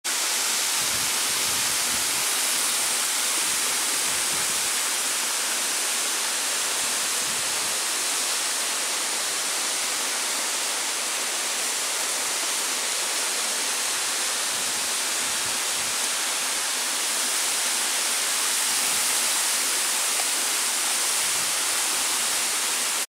Cascada Vanturatoarea
Una din cele mai mari cascade din Romania. Cand debitul apei e mai mare e absolut superba caderea de apa...